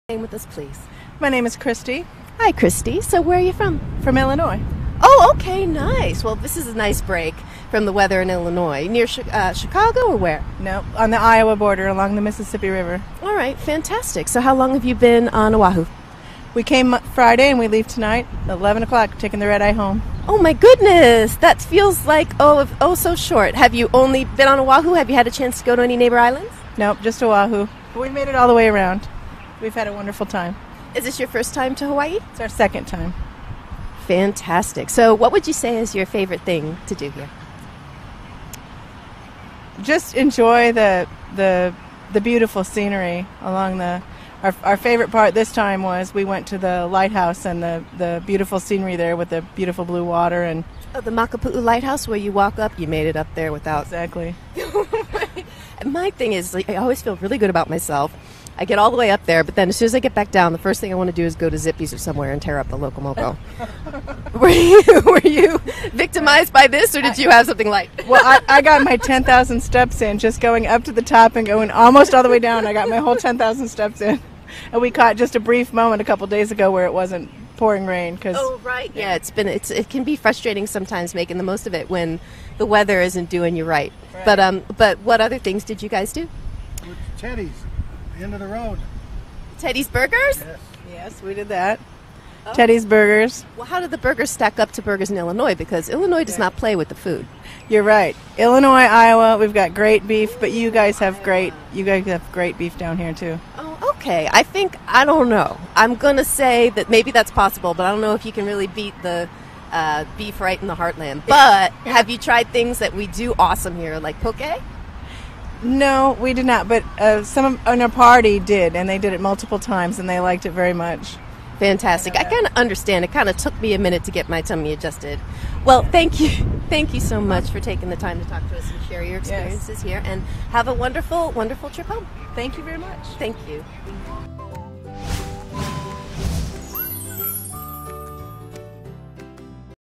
Vacationing in Oahu - The View from Honolulu
Spotted relaxing on vacation, two visitors share their experiences with Raya Salter.